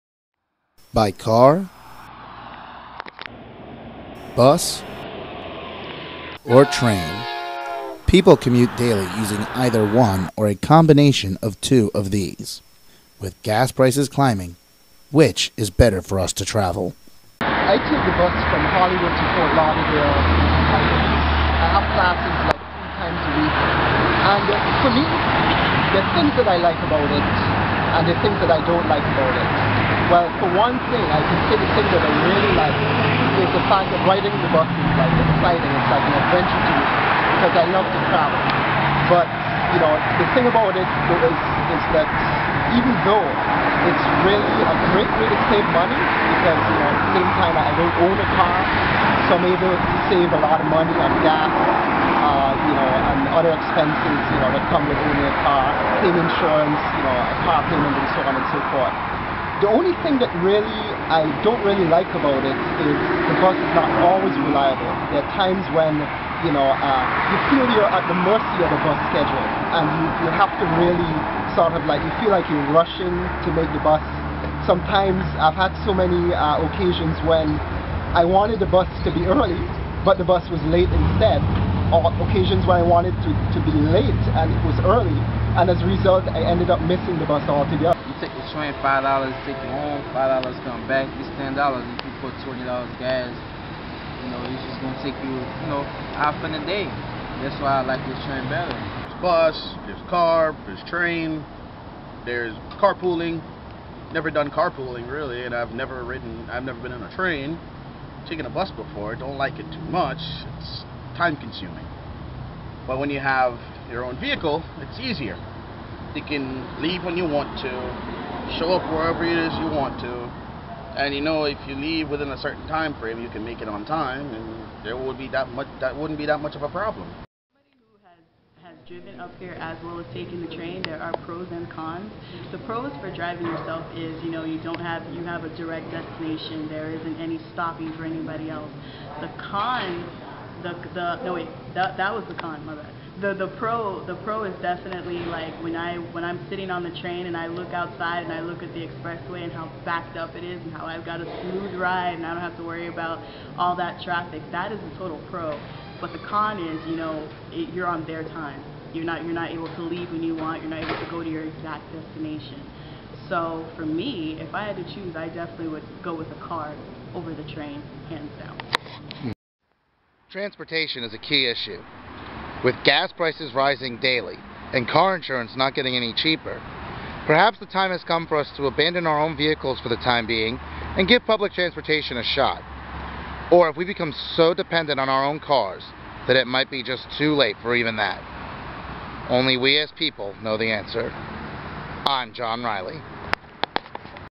Transportation Documentary - John Riley
Which is better for us to travel?  Here is the opinion of some who utilize a variety of ways of transportation.